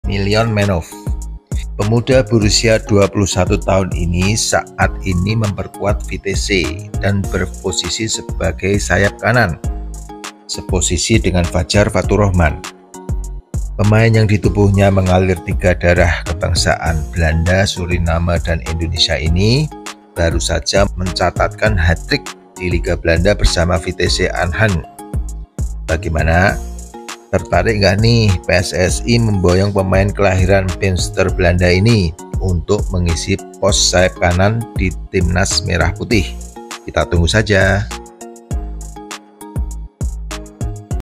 Million Manhoef Gabung Timnas: Bintang Baru Garuda? 🔥
Million Manhoef adalah pemain sepak bola Belanda-Indonesia yang bermain sebagai bek untuk klub Eredivisie Vitesse Arnhem. Dia adalah putra mantan petarung UFC Melvin Manhoef.

Manhoef lahir di Beemster, Belanda pada 3 Januari 2002. Dia mulai bermain sepak bola di usia muda dan dengan cepat naik pangkat di sistem pemuda Belanda. Dia melakukan debut profesionalnya untuk Vitesse pada tahun 2020 dan sejak itu telah membuat lebih dari 50 penampilan untuk klub tersebut.

Manhoef adalah bek serba bisa yang bisa bermain sebagai bek kiri dan bek kanan. Dia kuat di udara dan bagus dalam tekel. Ia juga pandai melakukan crossing dan memiliki passing range yang bagus.

Manhoef telah mewakili Belanda di level U-19 dan U-21. Dia juga dipanggil ke skuat senior Belanda, tapi belum tampil.

Manhoef berhak bermain untuk Indonesia melalui ibunya. Dia telah menyatakan keinginannya untuk bermain untuk Indonesia dan mengatakan bahwa dia bersedia untuk segera dinaturalisasi.

Jika Manhoef dinaturalisasi, dia akan menjadi dorongan besar bagi timnas Indonesia. Dia adalah bek berbakat yang akan menambah pengalaman dan kualitas tim. Dia juga akan menjadi panutan yang baik bagi pesepakbola muda Indonesia.